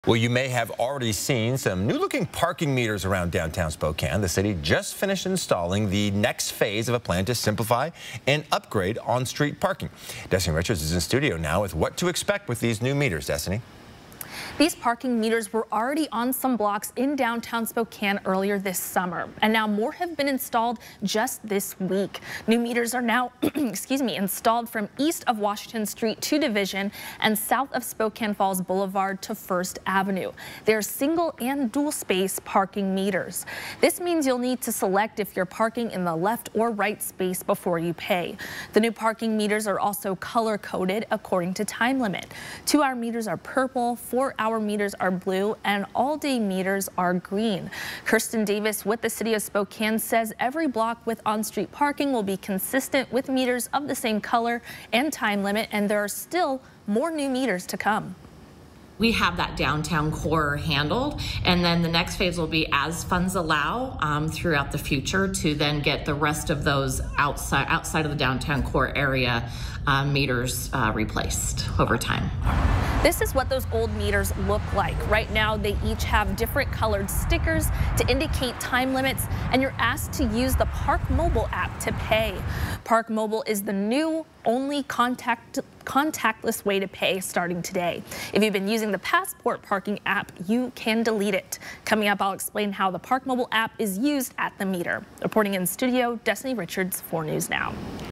New parking meters in downtown Spokane
Destiny Richards breaking down the next phase of the city's plan to simplify and upgrade street parking.